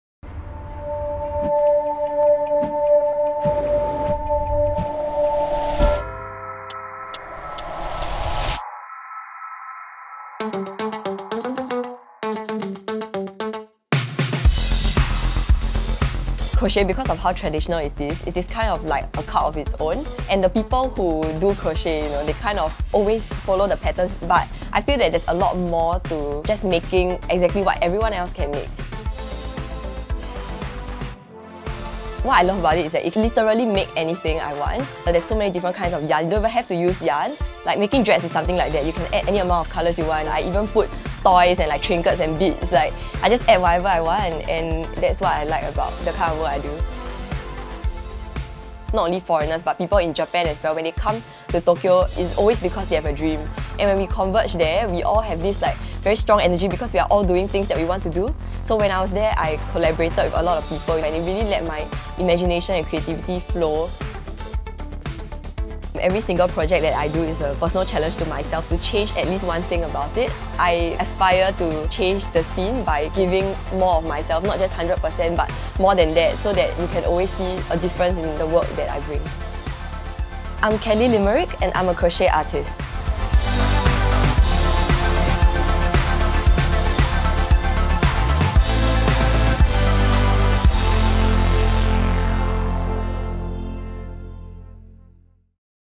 Louis Vuitton x L'Officiel: Time Stands Still for Kelly Limerick
Time stands still for Kelly Limerick @kllylmrck, a crochet artist whose dedication towards her craft reflects the true Louis Vuitton spirit.